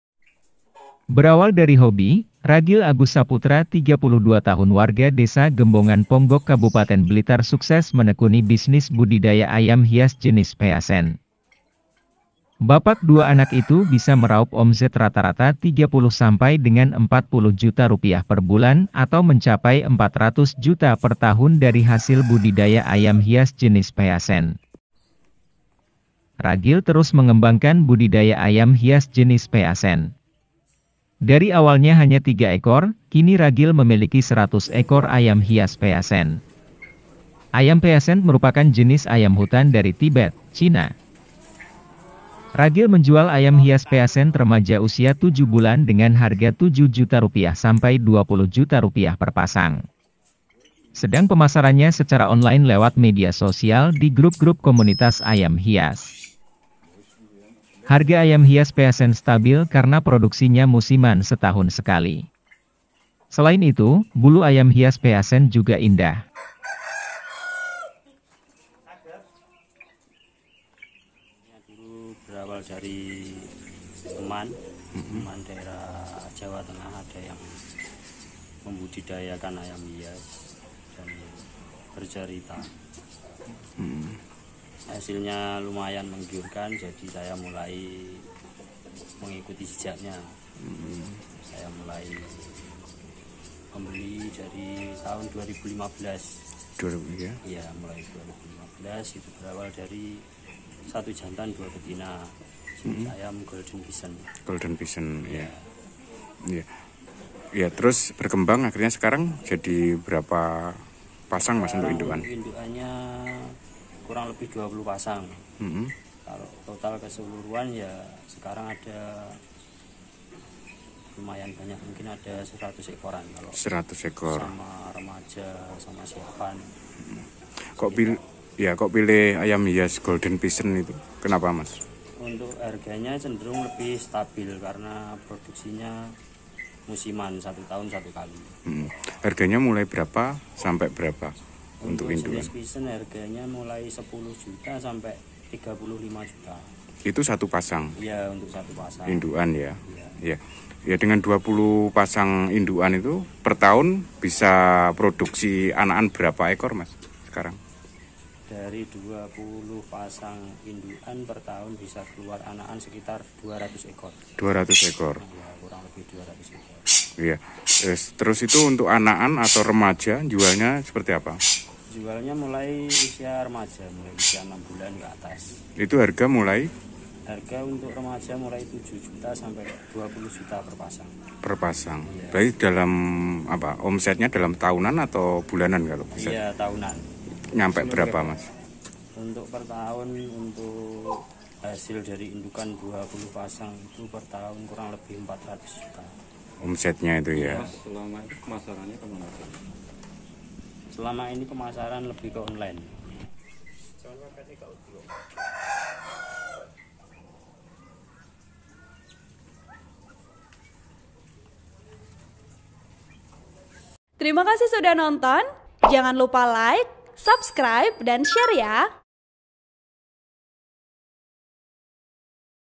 Budidaya Ayam Hias Jenis Pheasent, Pria di Blitar Raup Rp 400 Juta Per Tahun
TRIBUNJATIM, BLITAR - Berawal dari hobi, Ragil Agus Saputra (32), warga Desa Gembongan, Kecamatan Ponggok, Kabupaten Blitar, sukses menekuni bisnis budidaya ayam hias jenis pheasent.

Bapak dua anak itu bisa meraup omzet rata-rata Rp 30-Rp 40 juta per bulan atau mencapai Rp 400 juta per tahun dari hasil budidaya ayam hias jenis pheasent.

Reporter: Samsul Hadi
( Goes)